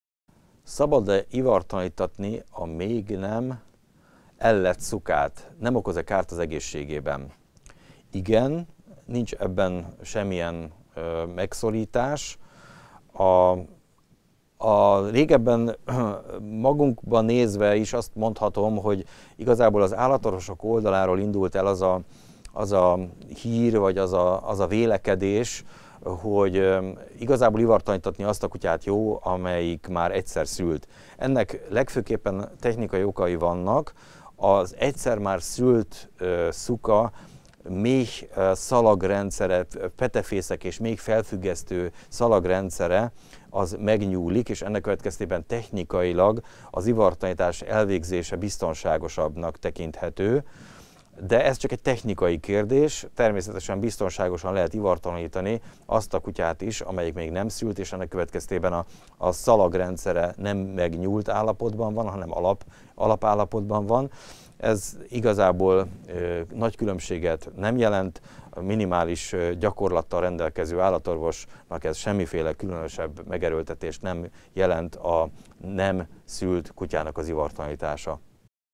Ivartalanítható e a még nem ellett szuka?
A legalább egyszer már ellett szuka méh- és petefészektartó szalagjai megnyúltabbak, így technikailag az ivartalanítás elvégzése nála biztonságosabb. De a szakorvosok tapasztalatai szerint gond nélkül lehet ivartalanítani a nem ellett szukát is.